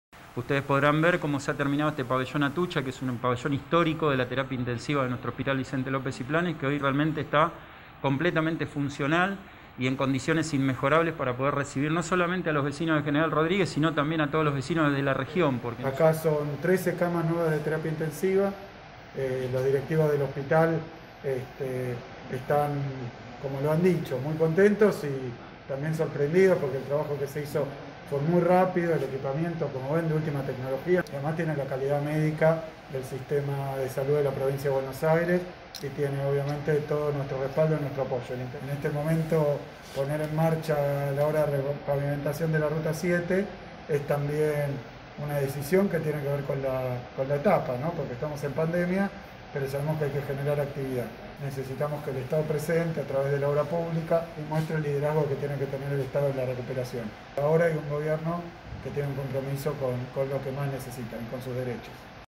Kicillof en la inauguración de la terapia intensiva del Hospital de General Rodríguez
El gobernador de la provincia de Buenos Aires, Axel Kicillof, junto al intendente de General Rodríguez, Mauro García, el ministro de Salud, Daniel Gollan, y el ministro de Infraestructura y Servicios Públicos, Agustín Simone, inauguraron este lunes una Unidad de Terapia Intensiva en el Hospital Interzonal General de Agudos Vicente López y Planes.